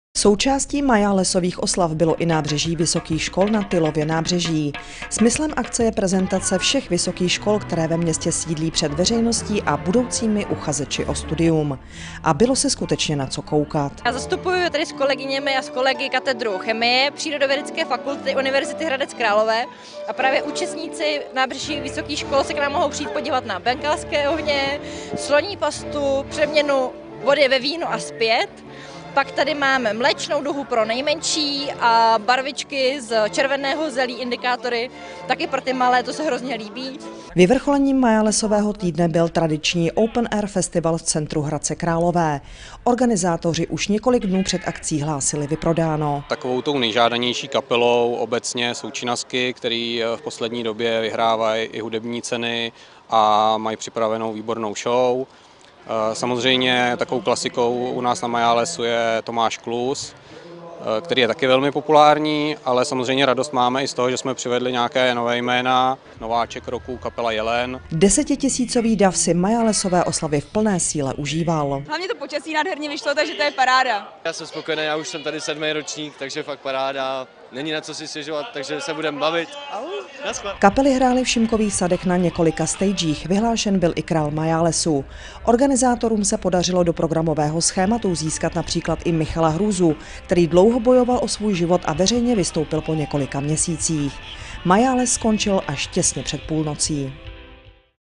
Majáles pobavil tisíce lidí
Celý týden probíhaly v Hradci Králové majálesové oslavy. O zábavu bylo skutečně postaráno a každý se vybral z velmi pestré nabídky akcí.